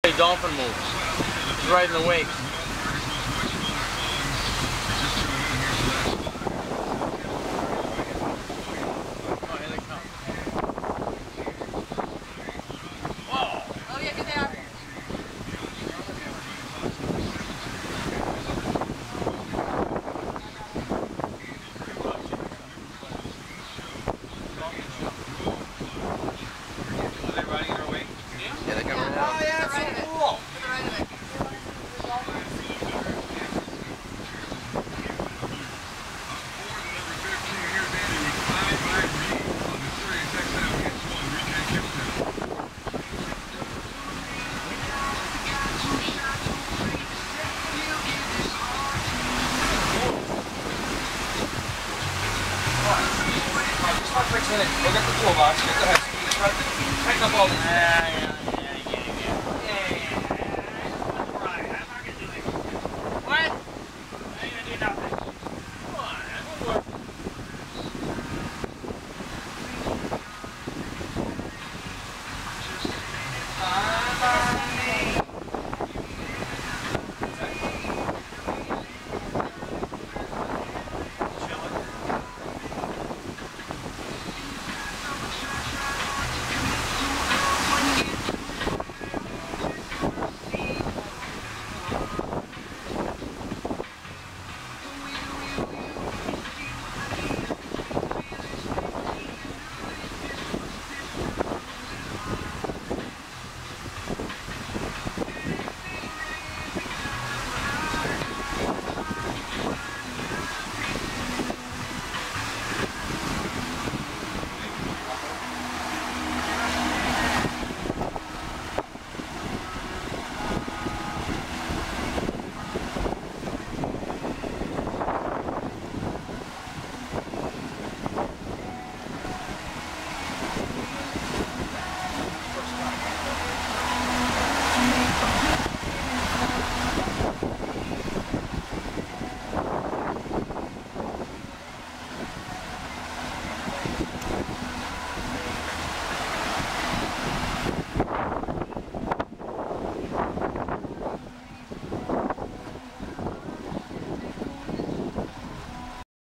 Dolphins following the yacht
Dolphins following the yacht in Jupiter FL